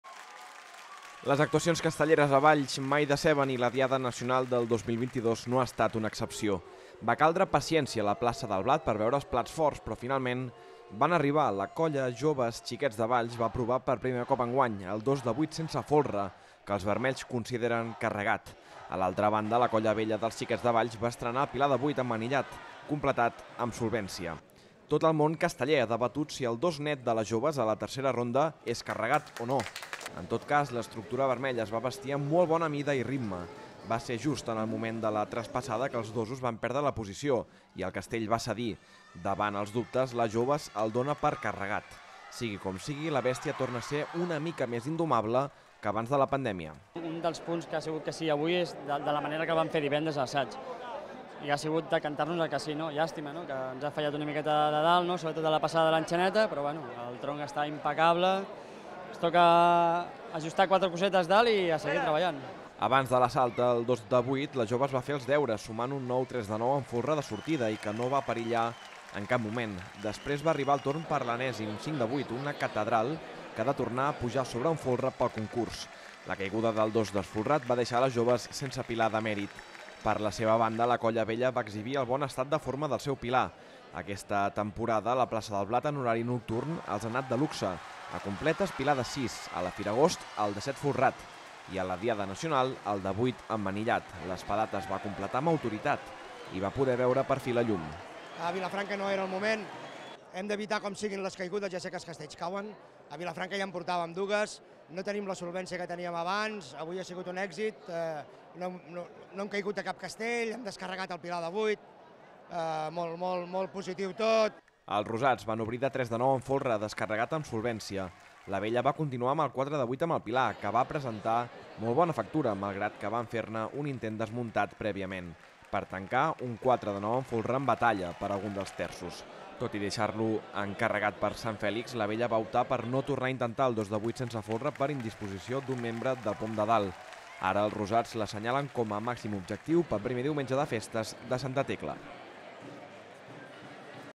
La Diada Nacional de Valls revalida la gamma extra
L'11 de setembre també és sinònim de castells a casa nostra. La vigília de la Diada Nacional, la plaça del Blat és l'escenari d'un nou duel vallenc. Tot i tenir una pandèmia pel mig, aquesta cita ha mantingut la tendència creixent dels últims anys i va revalidar la condició de gamma extra.